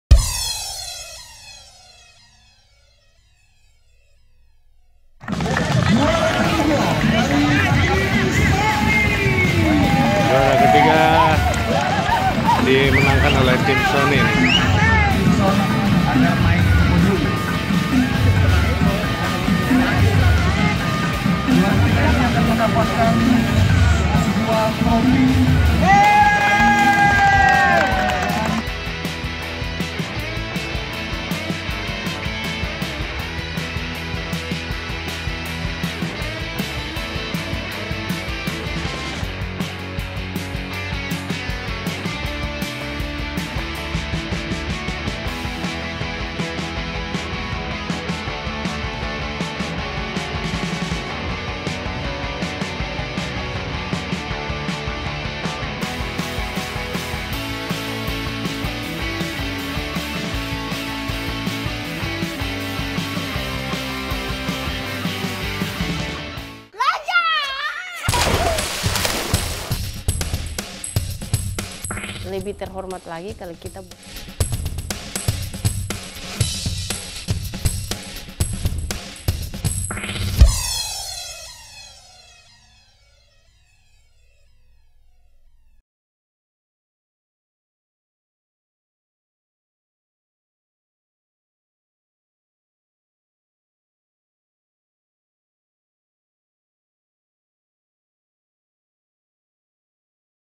SONY TAMPIL SBG JUARA 3 TURNAMEN FUTSAL PDG ELEKTRONIK CIHAPIT DI HUT TNI
turnamen futsall dalam rangka hut TNI